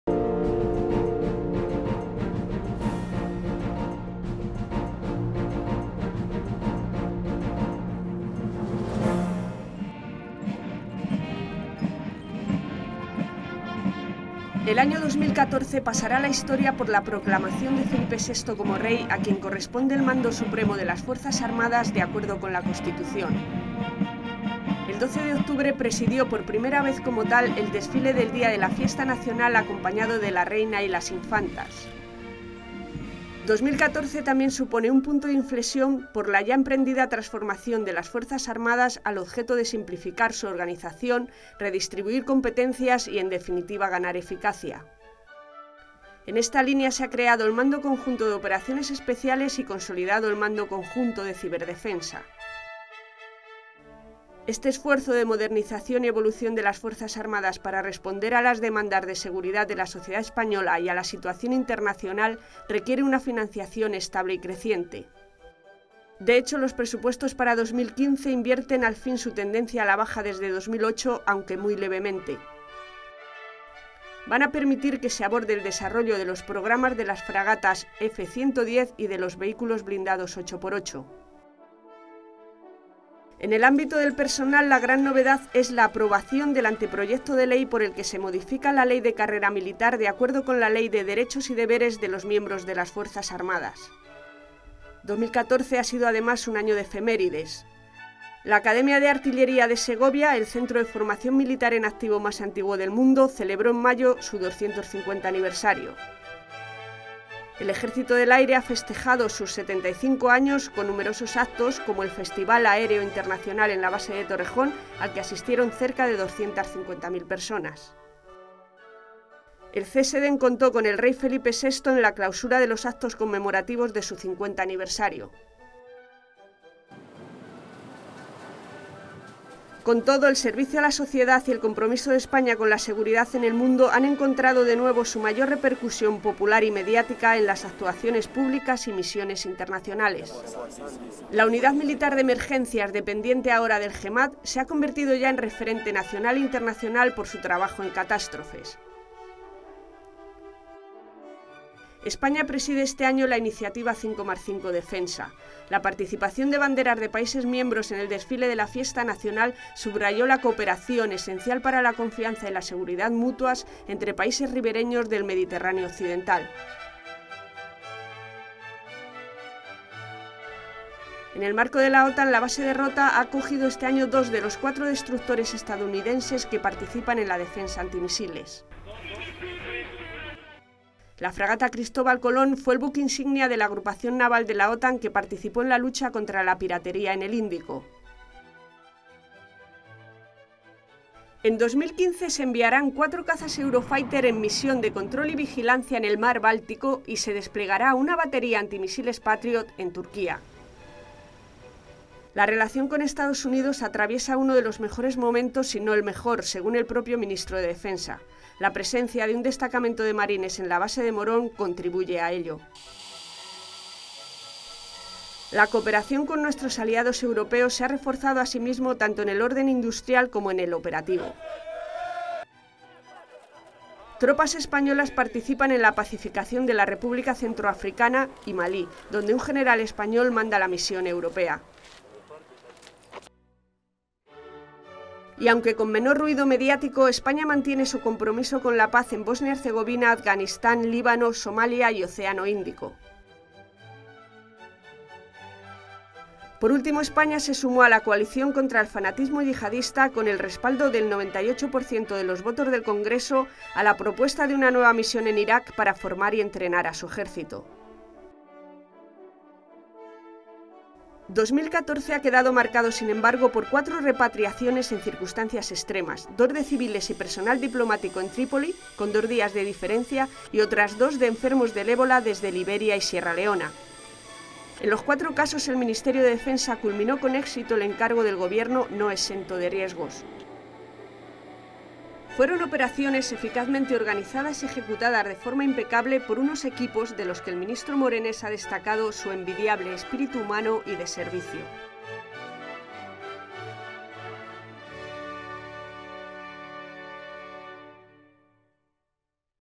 2014 Un año para recordar
El año 2014 pasará a la historia por la proclamación de Felipe VI como Rey, a quien corresponde el mando supremo de las Fuerzas Armadas de acuerdo con la Constitución.

El 12 de octubre presidió por primera vez como tal el desfile del Día de la Fiesta Nacional acompañado de la Reina y las Infantas.

2014 también supone un punto de inflexión por la ya emprendida transformación de las Fuerzas Armadas al objeto de simplificar su organización, redistribuir competencias y, en definitiva, ganar eficacia.

En esa línea, se ha creado el Mando Conjunto de Operaciones Especiales y consolidado el Mando Conjunto de Ciberdefensa.

Este esfuerzo de modernización y evolución de las Fuerzas Armadas para responder a las demandas de seguridad de la sociedad española y a la situación internacional requiere una financiación estable y creciente.

De hecho, los Presupuestos para 2015 invierten al fin su tendencia a la baja desde 2008, aunque muy levemente.

Van a permitir que se aborde el desarrollo de los programas de las fragatas F-110 y de los vehículos blindados 8x8.

En el ámbito del personal, la gran novedad es la aprobación del Anteproyecto de Ley por el que se modifica la Ley de Carrera Militar de acuerdo con la Ley de Derechos y Deberes de los miembros de las Fuerzas Armadas.

2014 ha sido además un año de efemérides.

La Academia de Artillería de Segovia, el centro de formación militar en activo más antiguo del mundo, celebró en mayo su 250 aniversario.

El Ejército del Aire ha festejado sus 75 años con numerosos actos, como el festival aéreo internacional en la base de Torrejón al que asistieron cerca de 250.000 personas.

El CESEDEN contó con el Rey Felipe VI en la clausura de los actos conmemorativos de su 50 aniversario.

Con todo, el servicio a la sociedad y el compromiso de España con la seguridad en el mundo han encontrado de nuevo su mayor proyección popular y mediática en las actuaciones públicas y misiones internacionales.

La Unidad Militar de Emergencias, dependiente ahora del JEMAD, se ha convertido ya en referencia nacional e internacional por su trabajo en catástrofes.

España preside este año la Iniciativa 5+5 Defensa. La participación de banderas de países miembros en el desfile de la Fiesta Nacional subrayó la cooperación, esencial para la confianza y la seguridad mutuas, entre países ribereños del Mediterráneo occidental.

En el marco de la OTAN, la base de Rota ha acogido este año a dos de los cuatro destructores estadounidenses que participan en la defensa antimisiles.

La fragata ‘Cristóbal Colón’ fue el buque insignia de la agrupación naval de la OTAN que participó en la lucha contra la piratería en el Índico.

En 2015 se enviarán cuatro cazas 'Eurofighter' y una fragata al mar Báltico, en misión de control y vigilancia, y se desplegará una batería antimisiles 'Patriot' en Turquía.

La relación con Estados Unidos, “atraviesa uno de los mejores momentos, si no el mejor", según el propio ministro de Defensa, como confirma la presencia de un destacamento de marines en la base de Morón.

La cooperación con nuestros aliados europeos se ha reforzado asimismo tanto en el orden industrial como en el operativo.

Tropas españolas contribuyen a la pacificación de la República Centroafricana y Malí, donde un general español manda la misión europea.

Aunque con menor ruido mediático, España mantiene su compromiso con la paz en Bosnia-Herzegovina, Afganistán, Líbano, Somalia y Océano Índico.

Por último, España se sumó a la coalición contra el fanatismo yihadista con el respaldo del 98 por ciento de los votos del Congreso a la propuesta de una nueva misión en Irak para formar y entrenar a su Ejército.

2014 ha quedado marcado sin embargo por cuatro repatriaciones en circunstancias extremas: dos de civiles y personal diplomático de Trípoli, con dos días de diferencia, y otras dos de enfermos del ébola desde Liberia y Sierra Leona.

En los cuatro casos, el Ministerio de Defensa culminó con éxito el encargo del Gobierno, no exento de riesgos.

Fueron operaciones eficazmente organizadas y ejecutadas de forma impecable por unos equipos de los que el ministro Morenés ha destacado su “envidiable espíritu humano y de servicio”.